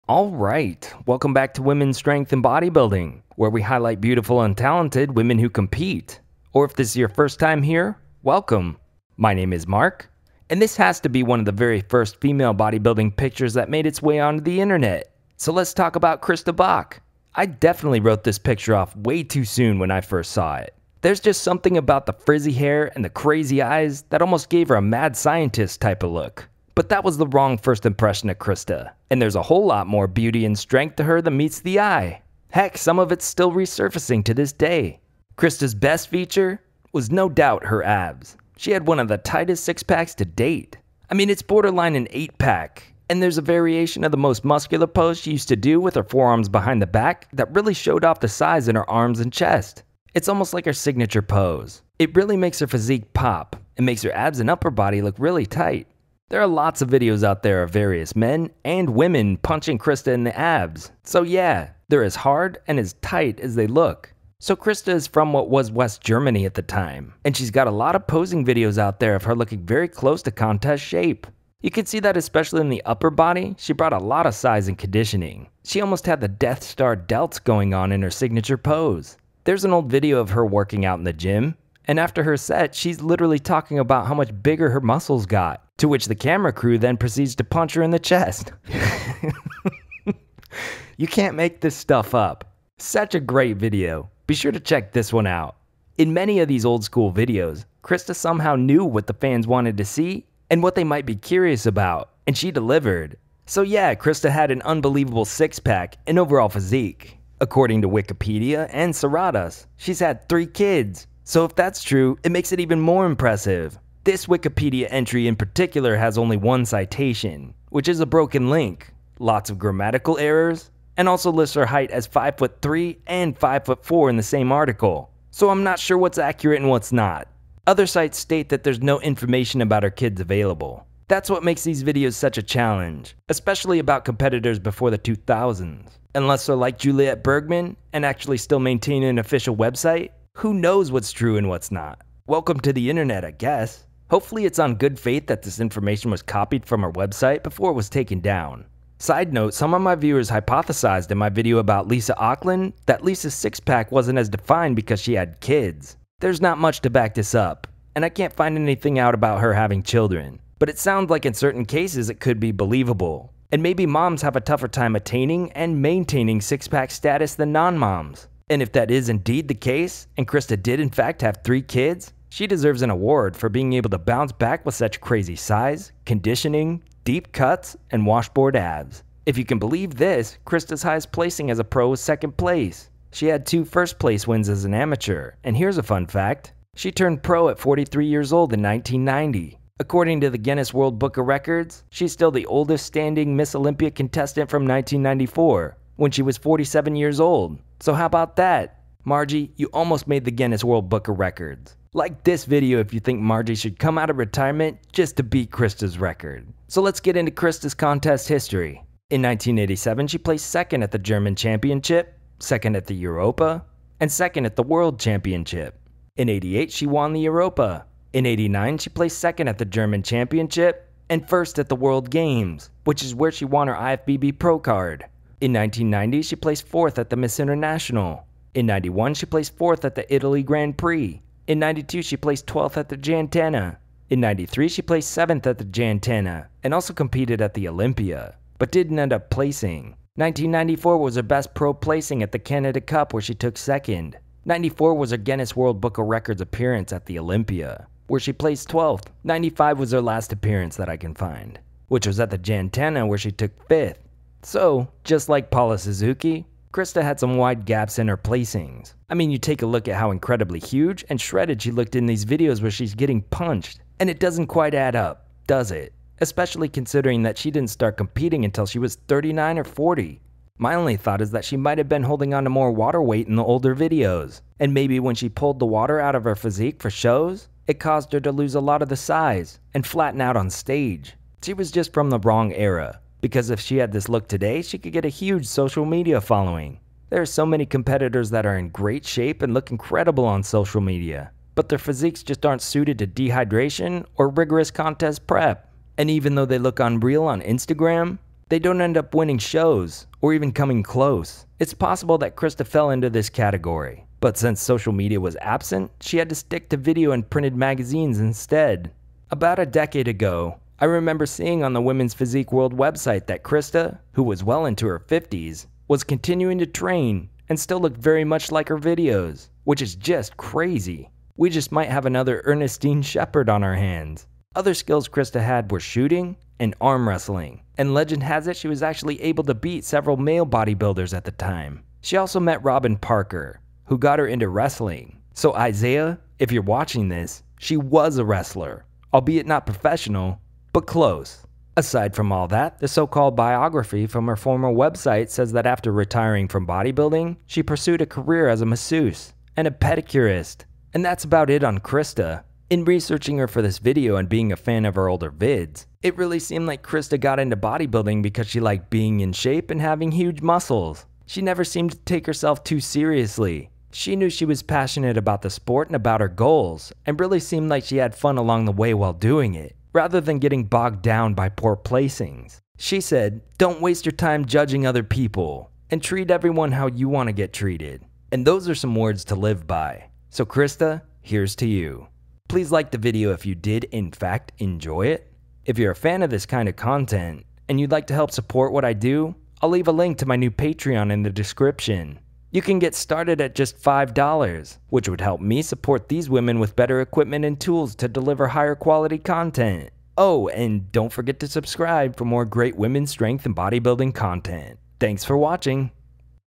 Christa Bauch Is In The Guinness World Book Of Records!
Today we’ll take a look at the career of the beautiful Christa Bauch! I’ll admit I wasn’t the biggest fan of hers on first glance, but after seeing how crazy her conditioning, structure and beauty was…she became one of my favorites! We’ll talk about where Christa is from, her funny & clever posing videos, her contest history, and what she’s been up to since retiring from bodybuilding!

Christa’s best feature was no doubt her abs. She had one of the tightest 6 packs to date. I mean it’s border line an 8 pack, and there’s a variation of the most muscular pose she used to do with her forearms behind the back that really showed off the size in her arms and chest. It’s almost like her signature pose, it really makes her physique pop and makes her abs and upper body look really tight.

There are lots of videos out there of various men and women punching her in the abs, so yeah…they’re as hard and as tight as they look.

So Christa is from what was West Germany at the time and she’s got a lot of posing videos out there of her looking very close to contest shape. You can see that especially in the upper body she brought a lot of size and conditioning. She almost had the “death star delts” going on in her signature pose. There’s an old video of her working out in the gym, and after her set she’s literally talking about how much bigger her muscles got, to which the camera crew then proceeds to punch her in the chest….you can’t make this stuff up. Such a great video, so be sure to check that one out.  In many of these old school videos, somehow Christa knew what fans wanted to see and what they might be curious about, and she delivered!

Yeah so Christa had an unbelievable 6 pack and overall physique. According to Wikipedia and Saradas, she’s had three kids, so if that’s true it makes it even more impressive. The Wikipedia entry in particular has only 1 citation which is a broken link, lots of grammatical errors, and also lists her height as 5’3” AND 5’4” in the same article. Other sites state that is no information about her kids available. That’s what makes these videos a challenge, especially about competitors before the 2000s. Unless they’re like Juliette Berman and actually still maintain an official website, who knows what’s true and what’s not. Welcome to the internet I guess. Hopefully it’s on good faith that this information was copied from her website before it was taken down.

Side note, some of my viewers hypothesized in my video about Lisa Aukland that Lisa’s 6 pack wasn’t as defined because she had kids. There’s not much to back this up, and I can’t find anything out about her having children, but it sounds like in certain cases it could be believable and maybe moms have a tougher time attaining and maintaining 6 pack status than non moms. And if that is indeed the case and Christa did in fact have 3 kids, then she deserves and award for being able to bounce back with such crazy size, conditioning, deep cuts, and washboard abs.

If you can believe this, Christa’s highest placing as a pro was 2nd place. She had 2 first place wins as an amateur and here’s fun fact: She turned pro at 43 years old in 1990! According to the Guinness word book of records she is still the oldest standing Ms. Olympia contestant from 1994, when she was 47 years old so how about that?

Margie, you almost made it in to the Guinness World Book of records. Like the video if you think Margie should come out of retirement just to beat Christa’s record.